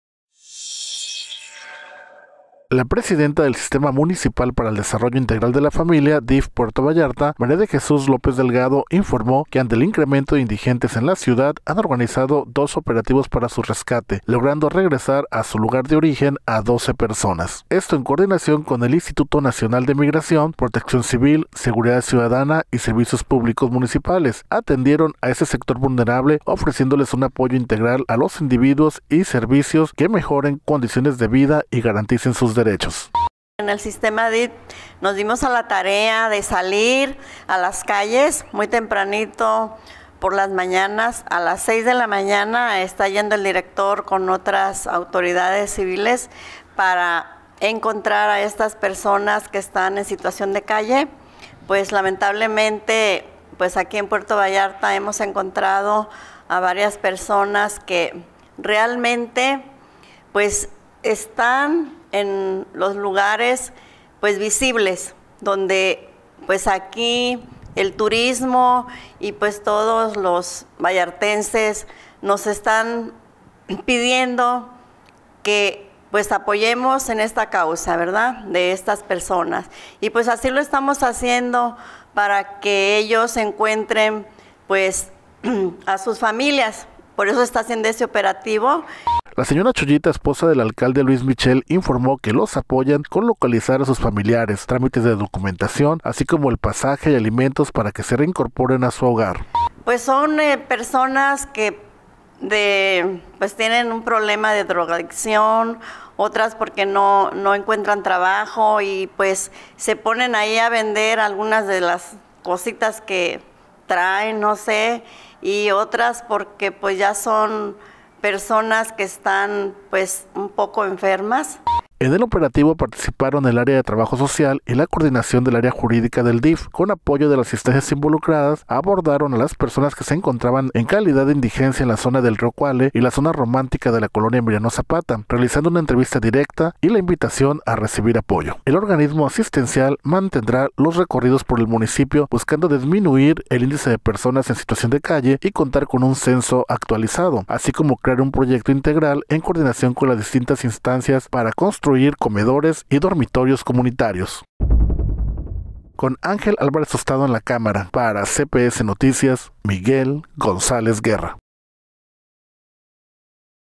DIF Vallarta ya regresó a 12 indigentes a su lugar de origen | CPS Noticias Puerto Vallarta
La presidenta del Sistema Municipal para el Desarrollo Integral de la Familia, DIF Puerto Vallarta, María de Jesús López Delgado, informó que ante el incremento de indigentes en la ciudad han organizado dos operativos para su rescate, logrando regresar a su lugar de origen a doce personas.